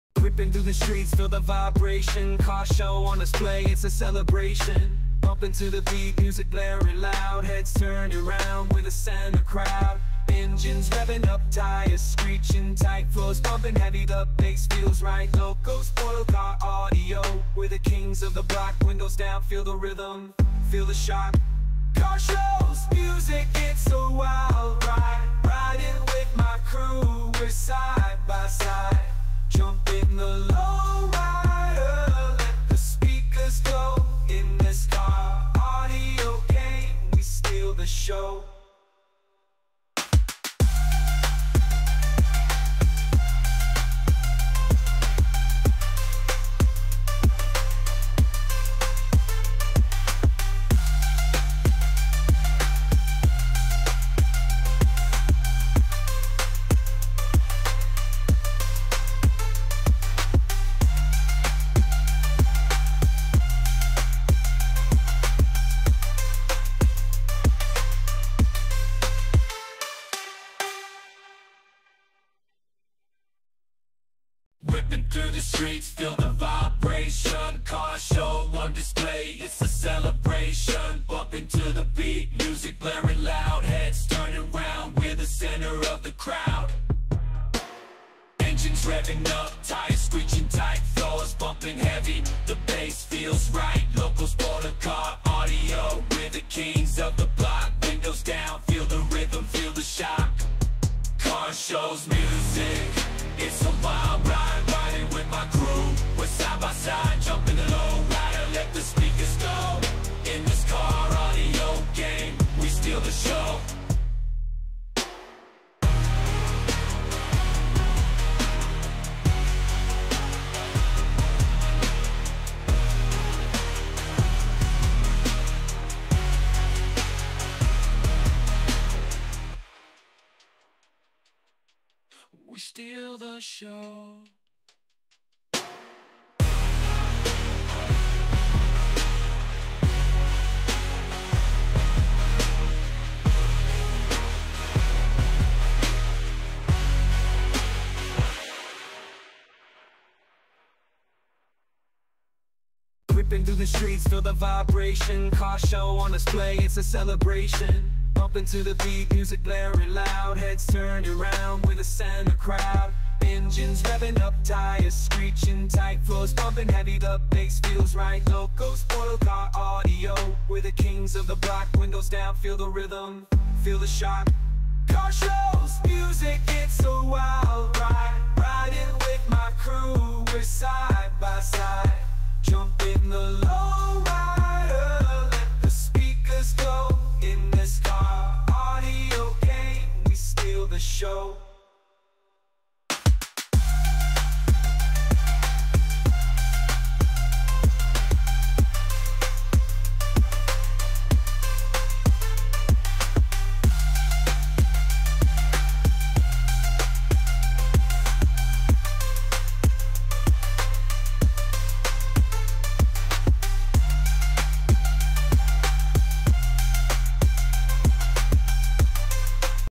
Predator Fest con Música creada con IA
Evento organizado por el Predator CAR CLUB de Pastor Ortiz, Michoacán.